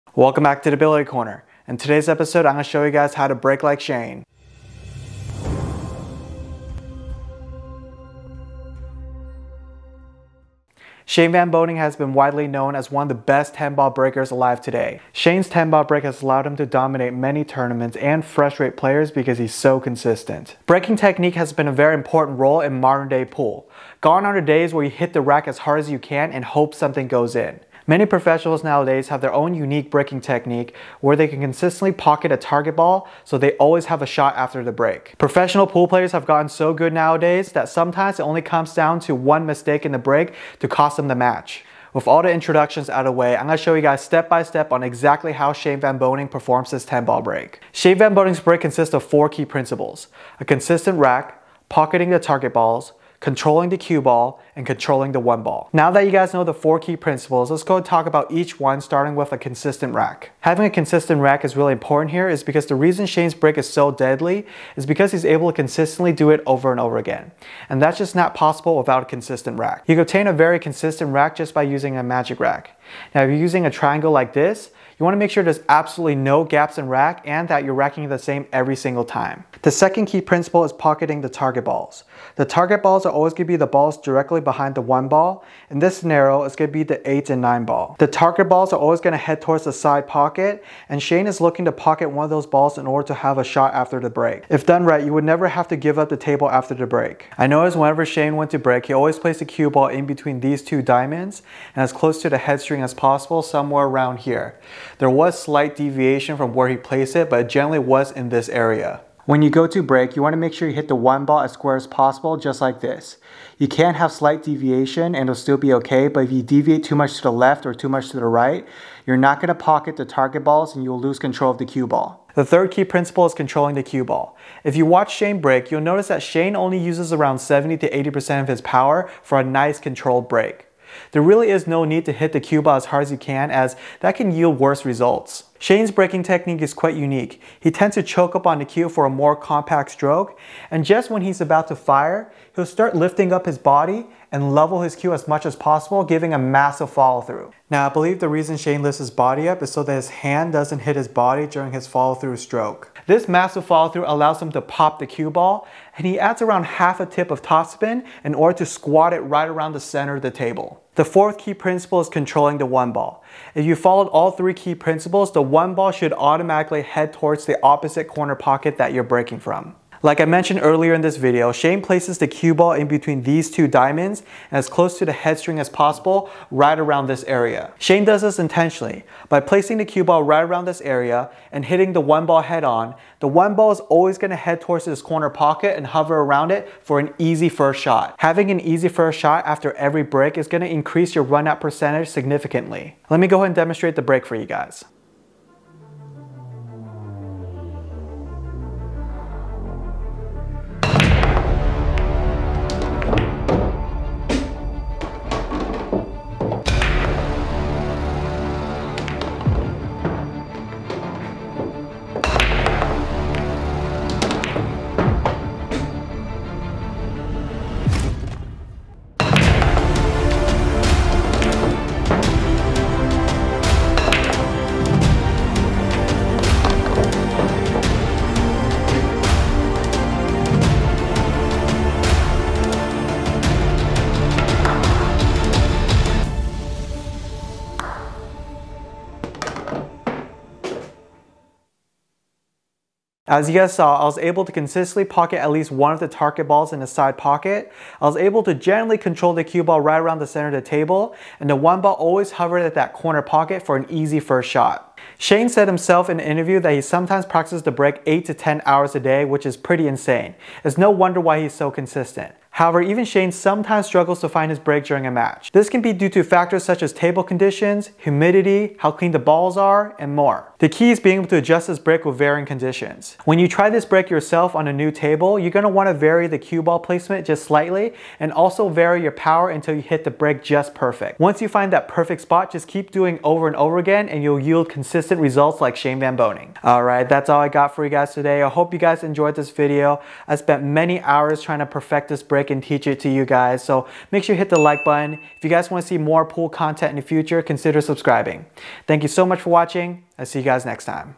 Shane Van Boening's 10-Ball Break Explained
Shane Van Boening has often been regarded as the best 10-ball breaker alive today. His break has allowed him to dominate tournaments and frustrate players because he's so consistent. In this video, I analyze and explain SVB's deadly 10-ball break.

0:00 - Introduction
0:47 - 4 Key Principles
0:55 - Key Principle #1: Consistent Rack
1:19 - Key Principle #2: Pocketing Target Balls
2:02 - Key Principle #3: Controlling The Cue Ball
2:40 - Key Principle #4: Controlling The 1 Ball
3:14 - SVB 10-Ball Break Demo
4:02 - Final Thoughts